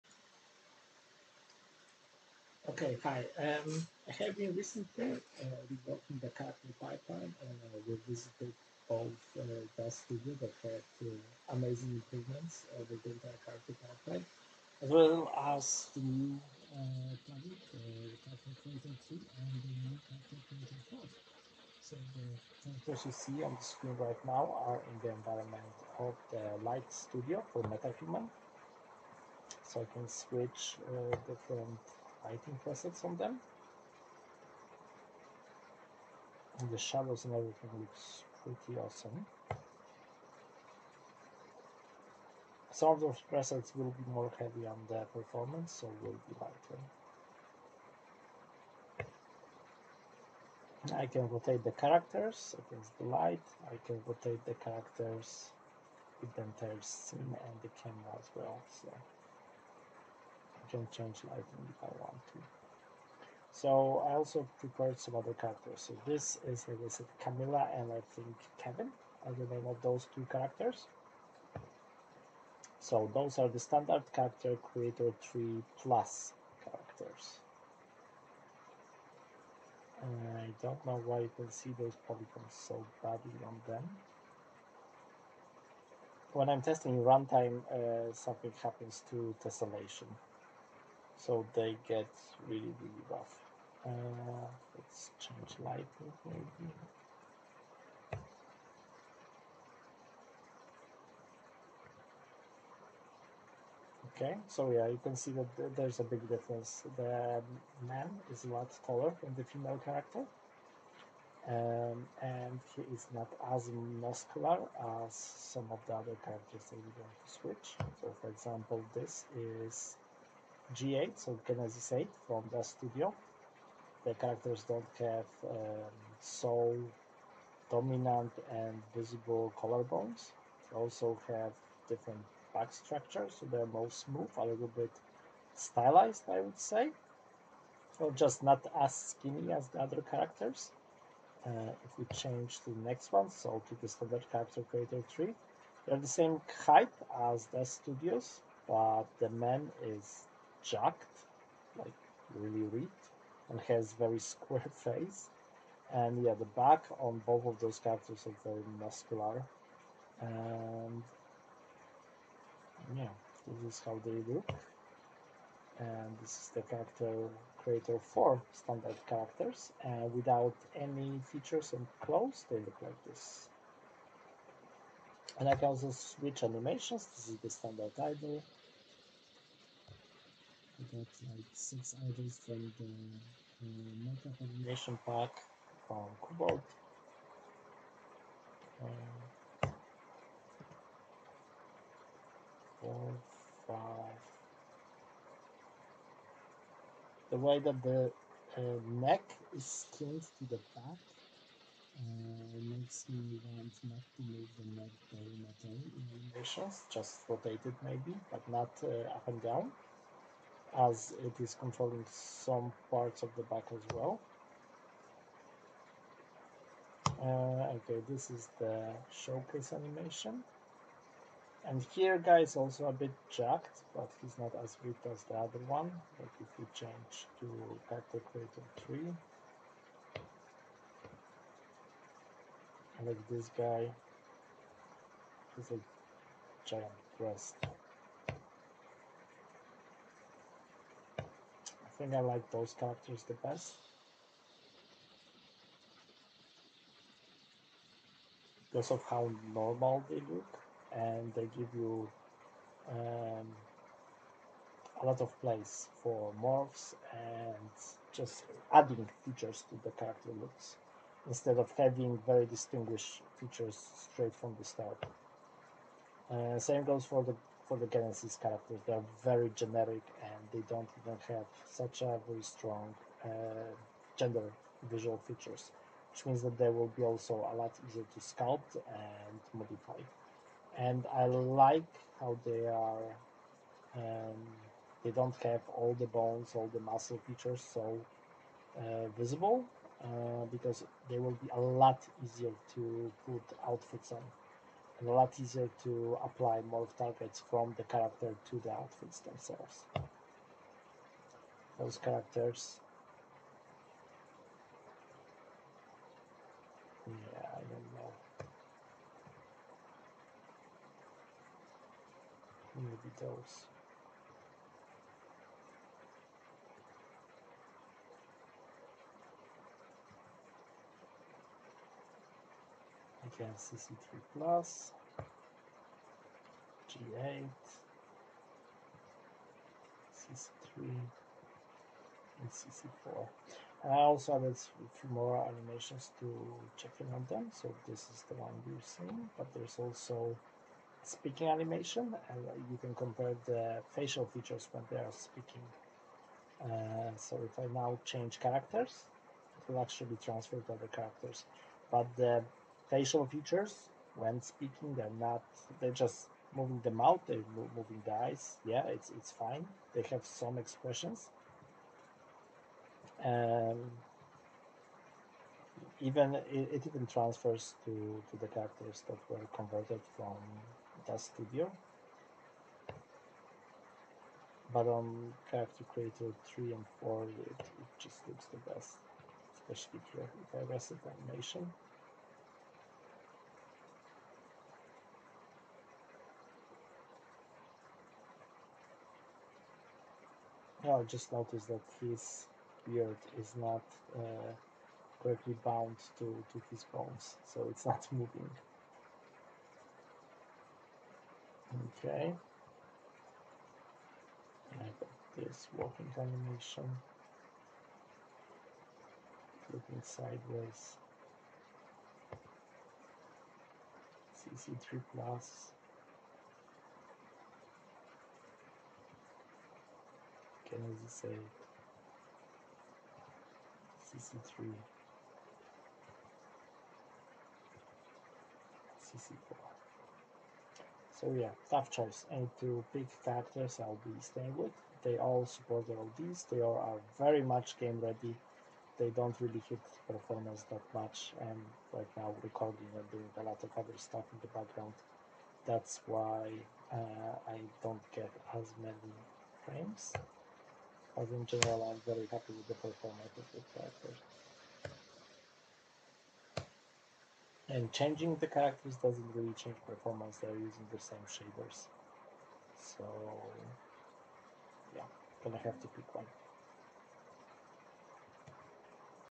CC3, CC4, DAZ Characters in Unreal 5
Working thru a pipeline to make a character creator with custom clothes and blendshapes in the unreal engine...
I will publish my findings and a short tutorial on best practices.